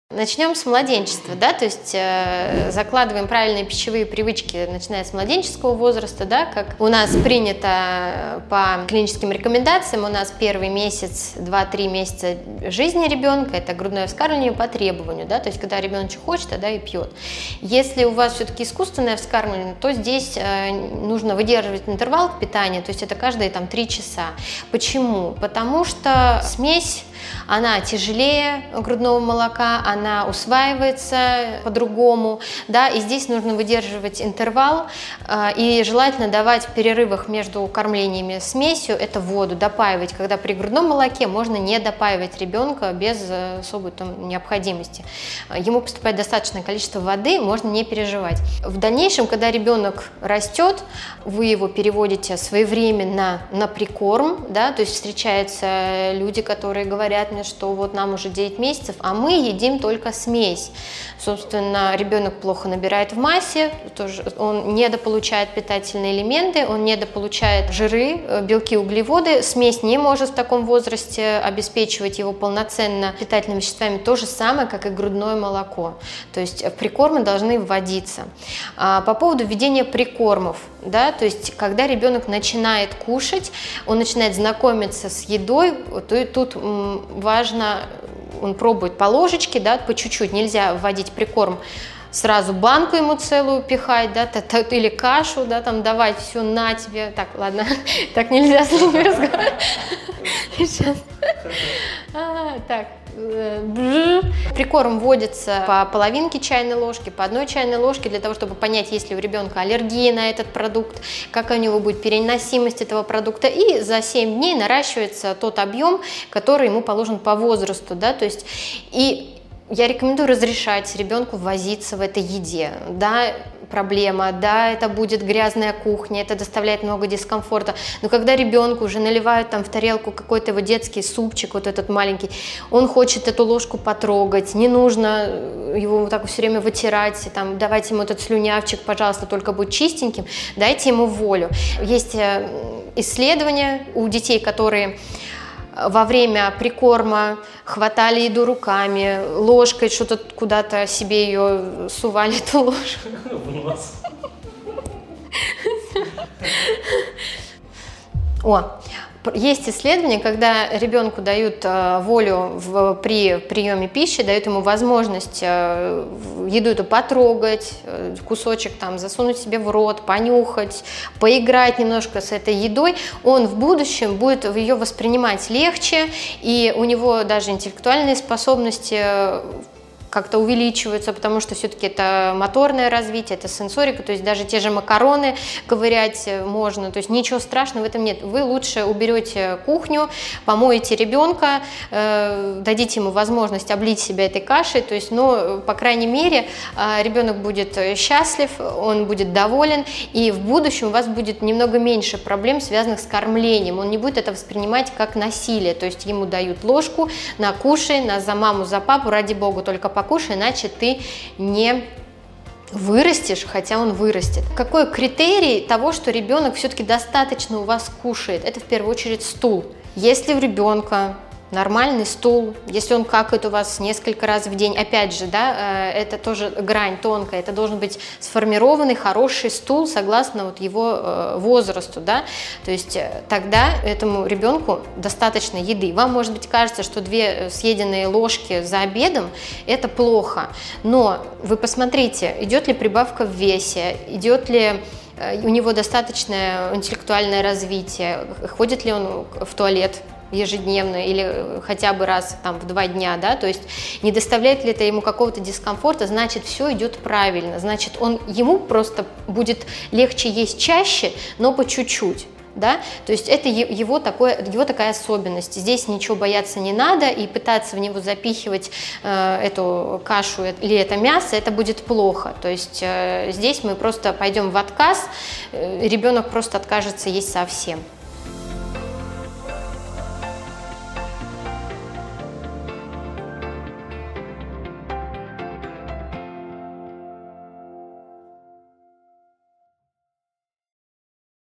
Грудное/искуственное вскармливание | Прикормы | Знакомство с едой | WMW
О том как правильно прививать ребенку любовь к еде рассказывает врач детский гастроэнтеролог клиники "Семейная" г. Тулы, Шкурина Дарья Владимировна.

*Instagram является продуктом Meta, деятельность которой запрещена в РФ